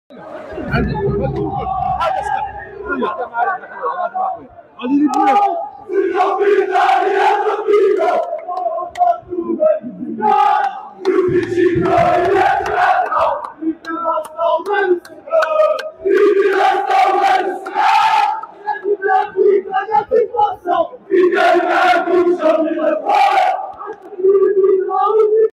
🟢شاهد... بكاء وتأثر سليم براهيمي لاعب أمل الأربعاء بعد نهاية مباراة فريقه ضد رائد القبة التي عرفت ت
🟢شاهد... بكاء وتأثر سليم براهيمي لاعب أمل الأربعاء بعد نهاية مباراة فريقه ضد رائد القبة التي عرفت تحقيقهم للبقاء في الأنفاس الاخيرة من المباراة